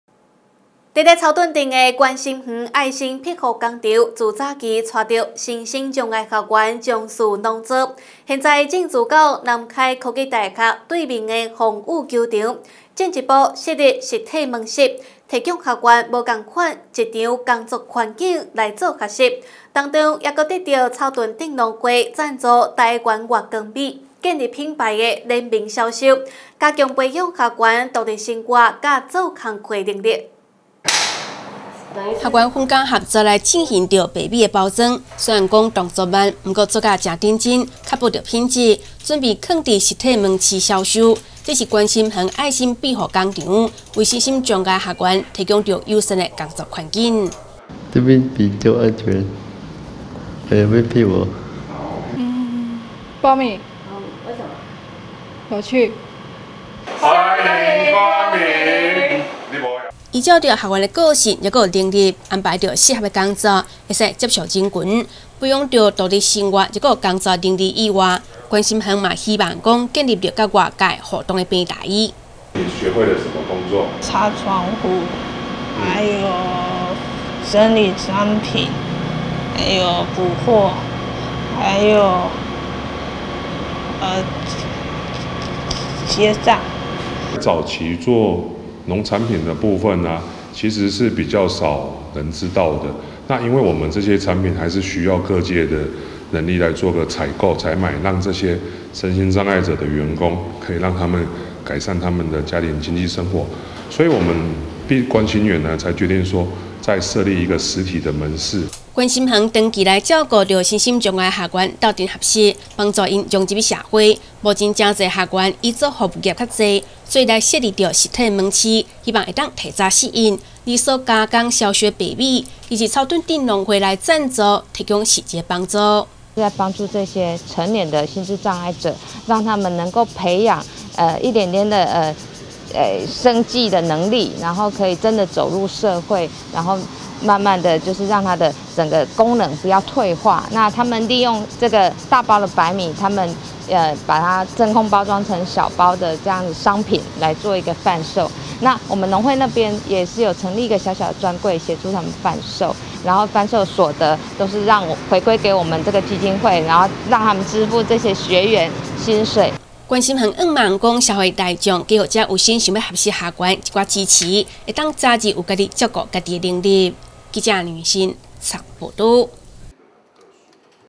觀心園設實體門市 培養身心障礙學員工作能力 南投新聞
草屯鎮的觀心園愛心庇護工場，從早期帶著身心障礙學員從事農作，現在進駐到南開科技大學對面的風雨球場，進一步設立實體門市，提供學員不同的職場工作環境來學習，當中，還獲得草屯鎮農會贊助台灣越光米，建立品牌聯名銷售，加強培養學員獨立生活和工作的能力。